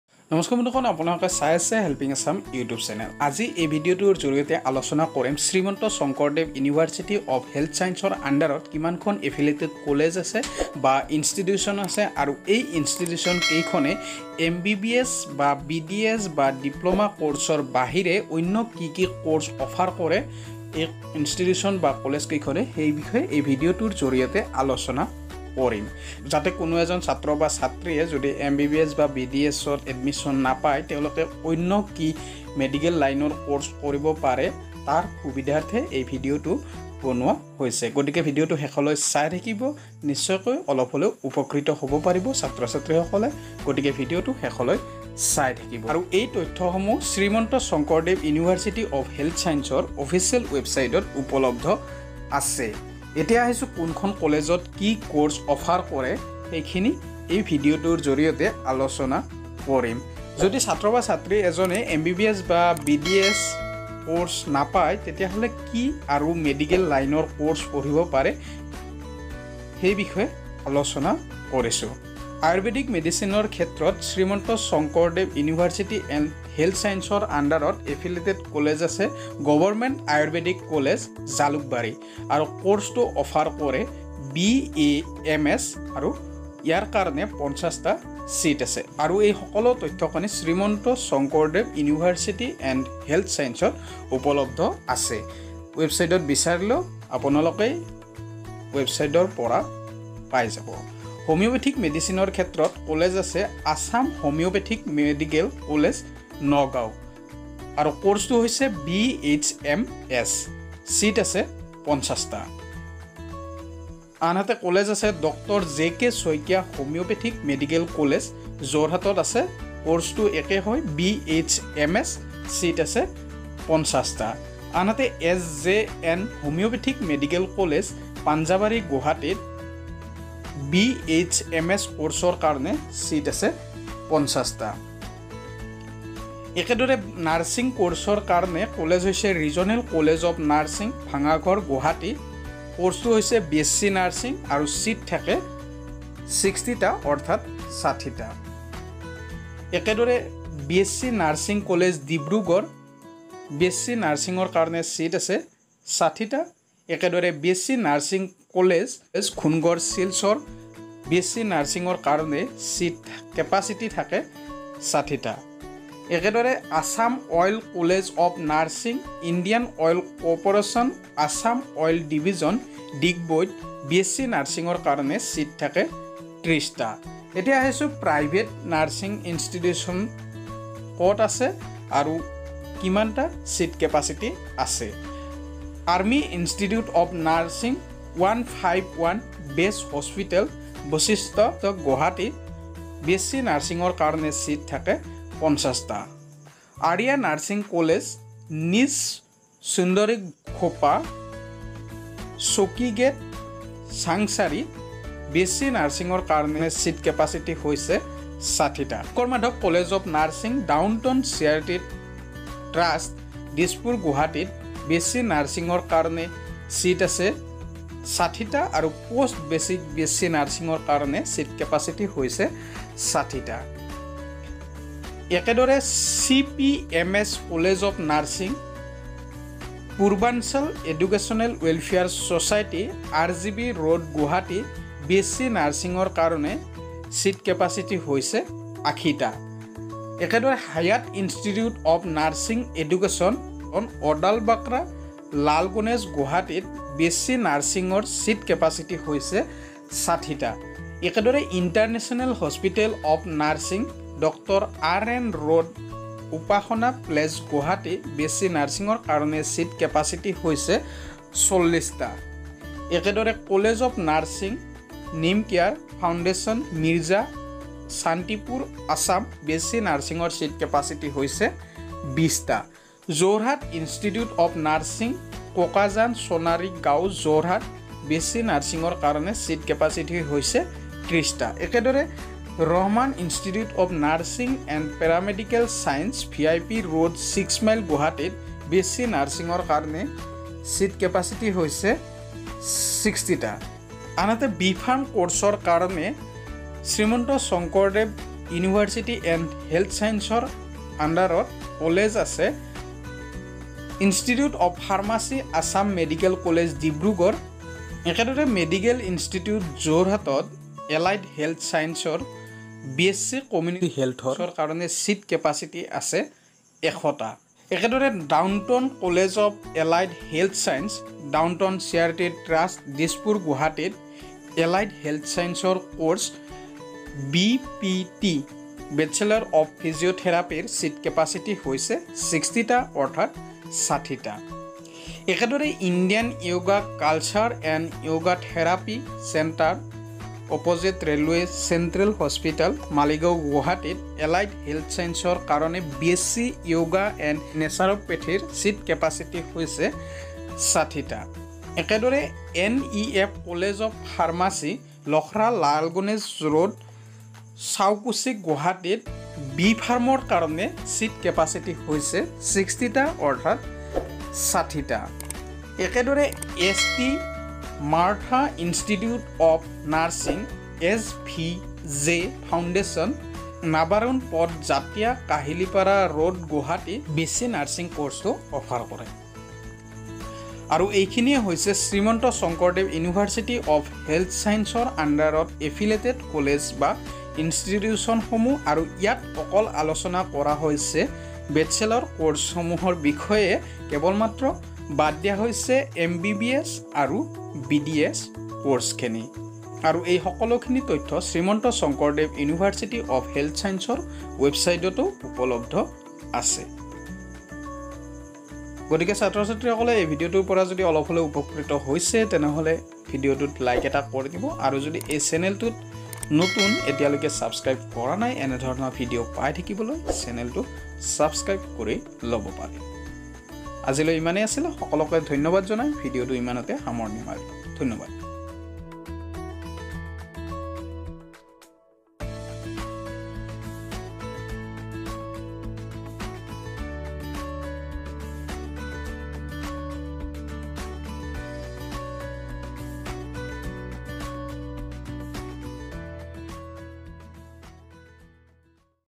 BAMS, BHMS, BSc Nursing, BPT, B Pharm Course in Assam | Seats Capacity | SSUHS, Assam Admission 2020
SRIMANTA SANKARADEVA UNIVERSITY OF HEALTH SCIENCES, Assam

Name of College/Institute
Course
Seat Capacity